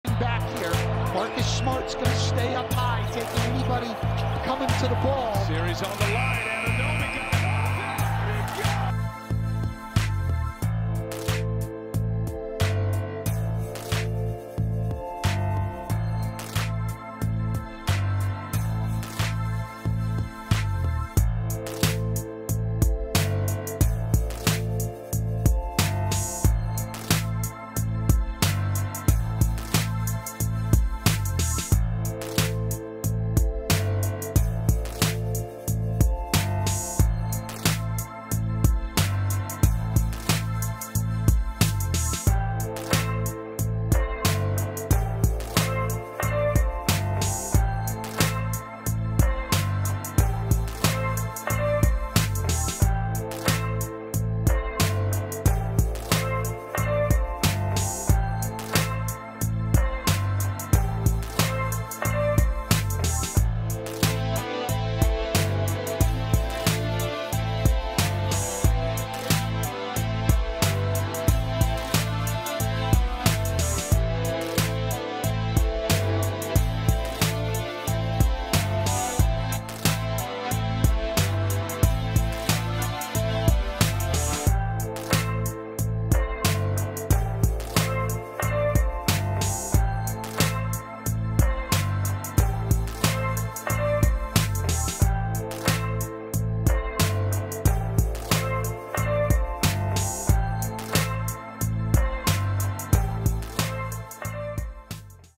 OG Anunoby Winning shot..but does it really count?! | Raptors vs Celtics Game 3
OG Anunoby Winning shot..but does it really count?! | Toronto Raptors vs Boston Celtics Game 3 East Round 2 NBA 2020 Playoffs

All videos are made with the intent of learning and promoting NBA.